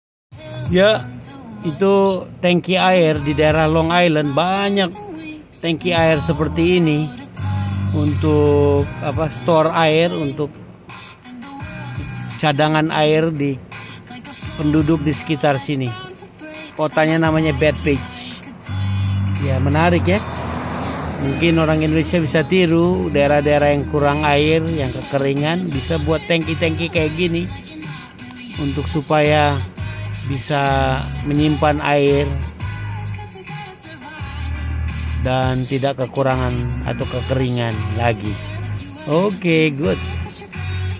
Water tank in Long Island, NYC
giant water tank to store water , so the residences not lack of water